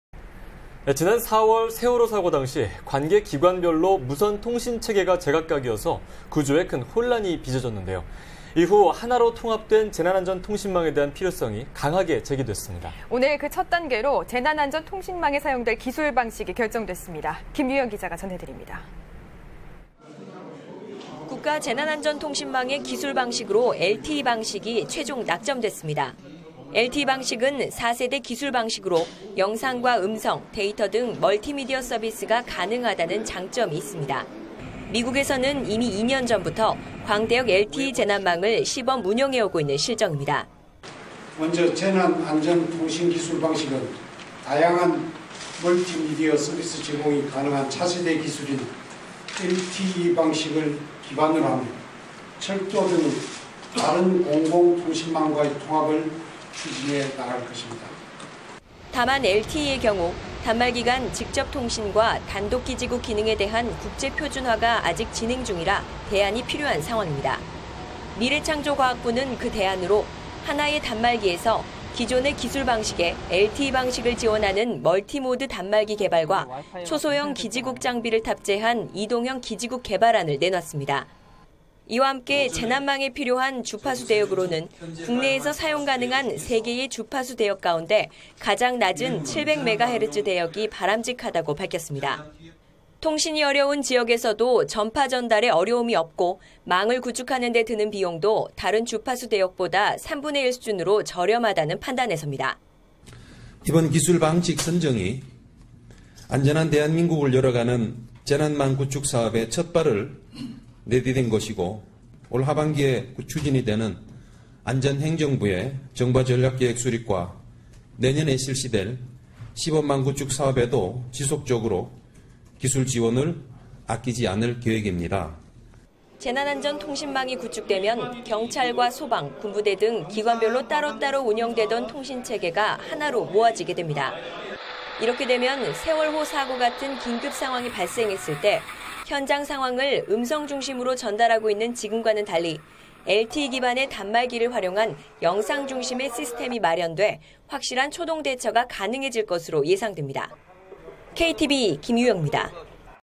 [KTV] "국가재난안전통신망 LTE 방식"…통합통신망 '가속'
"국가재난안전통신망 LTE 방식"…통합통신망 '가속'

지난 4월 세월호 사고 당시, 관계 기관별로 무선통신체계가 제각각이어서 구조에 큰 혼란이 빚어졌는데요.
이후 하나로 통합된 재난안전통신망에 대한 필요성이 강하게 제기됐습니다. 오늘 그 첫 단계로 재난안전통신망에 사용될 기술방식이 결정됐습니다.